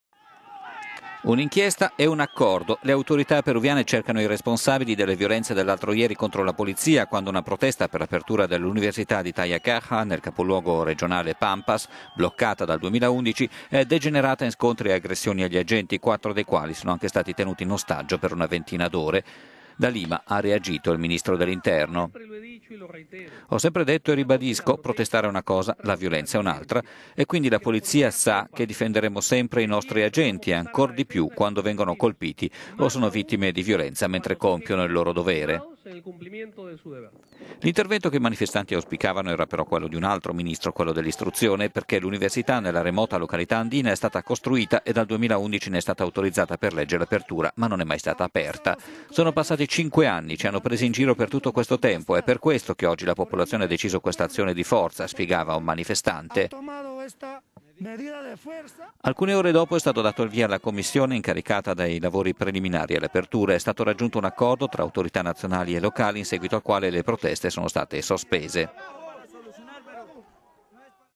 Perù: dopo violenti scontri, via alla nuova Università andina
Un'inchiesta e un accordo: le autorità peruviane cercano i responsabili delle violenze dell'altro ieri contro la polizia, quando una protesta per l'apertura dell'Università di Tayacaja, nel capoluogo regionale Pampas, bloccata dal 2011, è degenerata in scontri e aggressioni agli agenti, quattro dei quali sono anche stati tenuti in ostaggio per una ventina d'ore.

Da Lima ha reagito il Ministro dell'Interno:
"Ho sempre detto e ribadisco: protestare è una cosa, la violenza è un'altra. E quindi l…